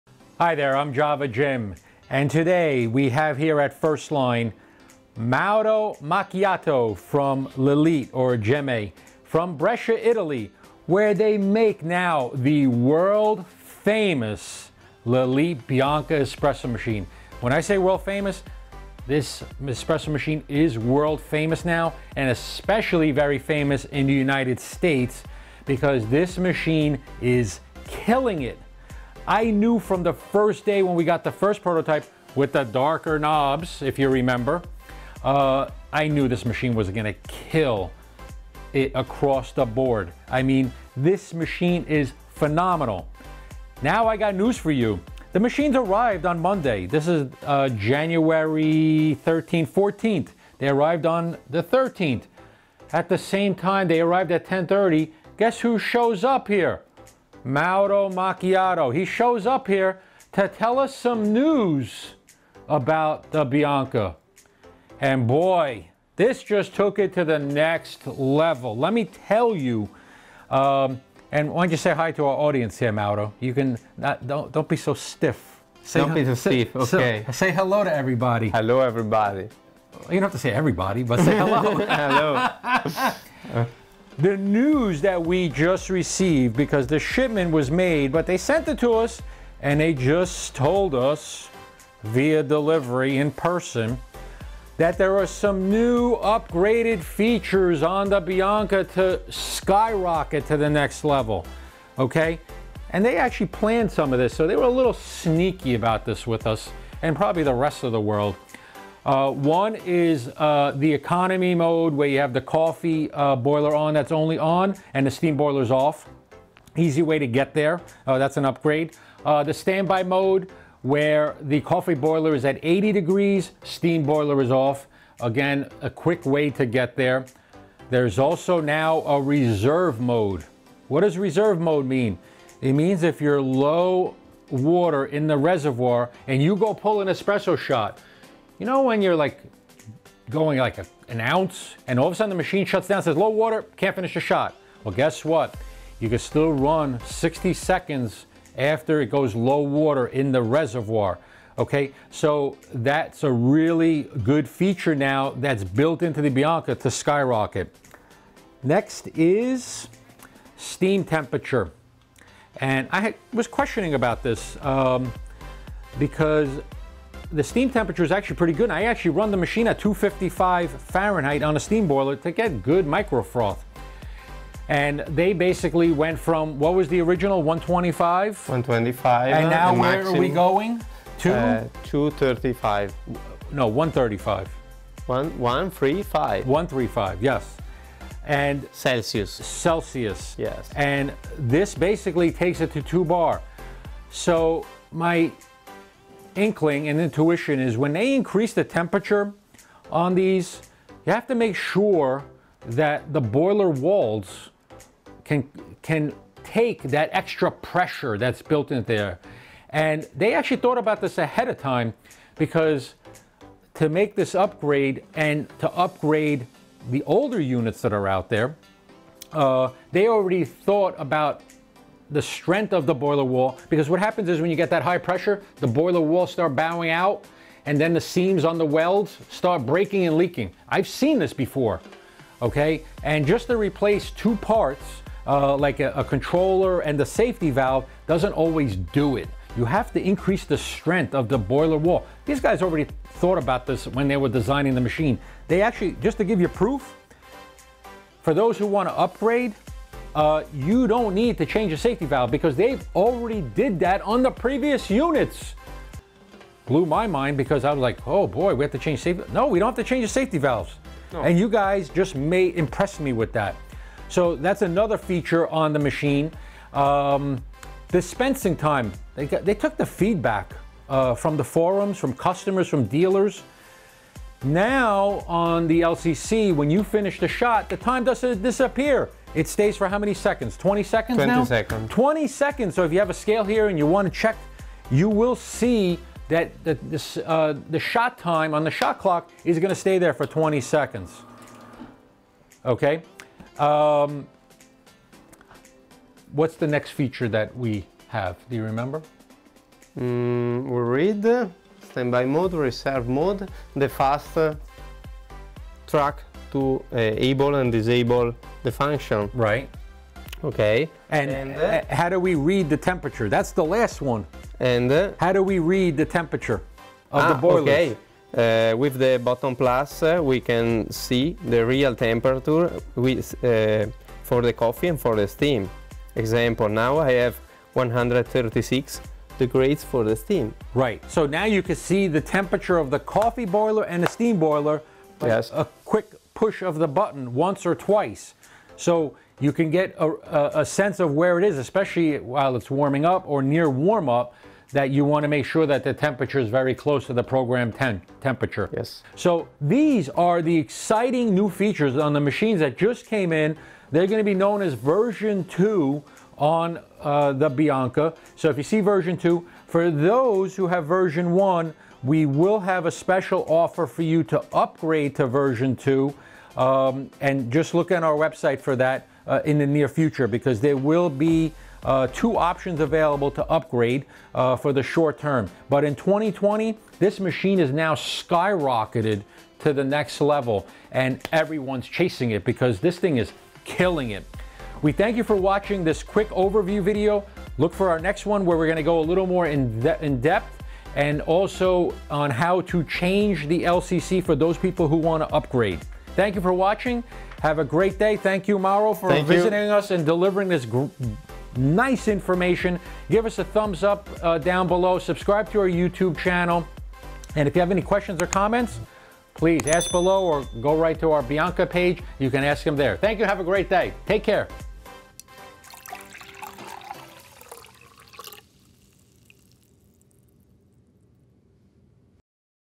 Lelit Bianca Version Upgrades: Learn All the New Features
Mauro Macchiato from Lelit and JavaJim explain all the upgrades that have arrived in Version 2 of the Lelit Bianca dual boiler espresso machine with flow control of brew pressure through a manual pressure profiling paddle.